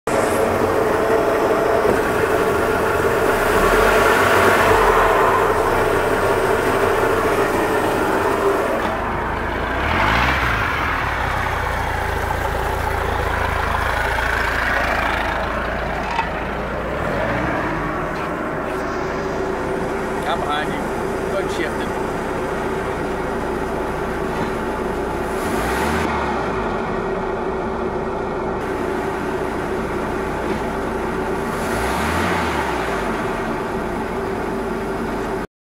Glastron Shift Issues Final Running Video 8 17 18
This is a boat that came in that quit shifting on the lake. The shift cable from the control box to the shift block on the engine was so tight it would not move. Now it seems to be shift OK.
 It is the original inner shift cable from the factory, and it does not shift perfectly since it shows to be slightly stiff. Also that roaring noise is the gimbal bearing that I bet has never been changed.